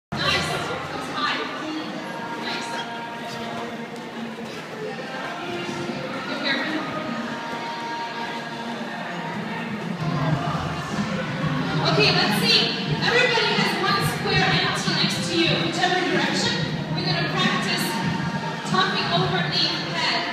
Sky Zone Skyrobics
Our first time trying Skyrobics! Super fun and its pretty challenging jumping all hour. They also have Sky hoops, foam pit, dodge ball and freestyle jumping all on trampoline courts. Very cool, I love it!